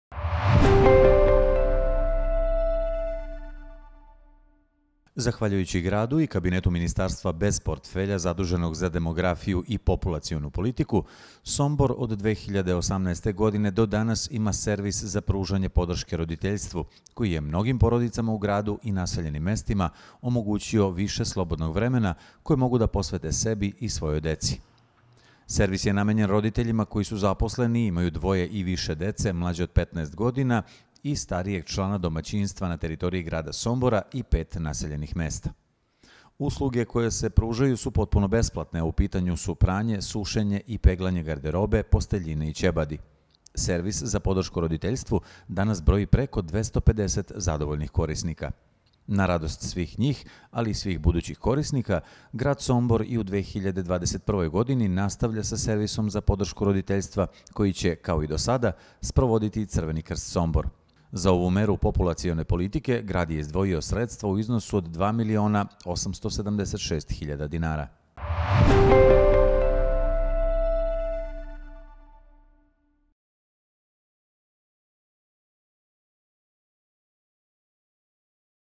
Servis podrške roditeljstvu biće nastavljen i u ovoj godini
Zahvaljujući Gradu i Kabinetu Ministarstva bez portfelja zaduženog za demografiju i populacionu politiku, Sombor, od 2018. godine do danas, ima Servis za pružanje podrške roditeljstvu, koji je mnogim porodicama u gradu i naseljenim mestima omogućio više slobodnog vremena koje mogu da posvete sebi i svojoj deci. Servis je namenjen roditeljima koji su zaposleni i imaju dvoje i više dece mlađe od 15 godina i/ili starijeg člana domaćinstva na teritoriji grada Sombora i 5 naseljenih mesta.